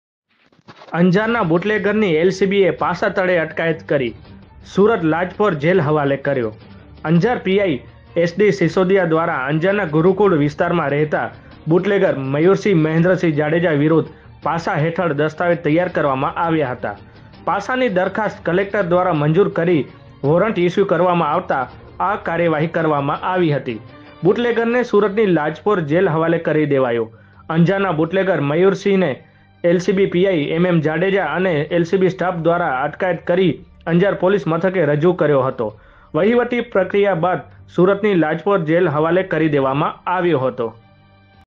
અંજારના બુટલેગરની પાસા તળે અટકાયત કરતી લોકલ ક્રાઇમ બ્રાન્ચ પુર્વ કચ્છ ગાંધીધામ
‎સત્ય પ્રેમ કરુણા તરફથી વિડિયો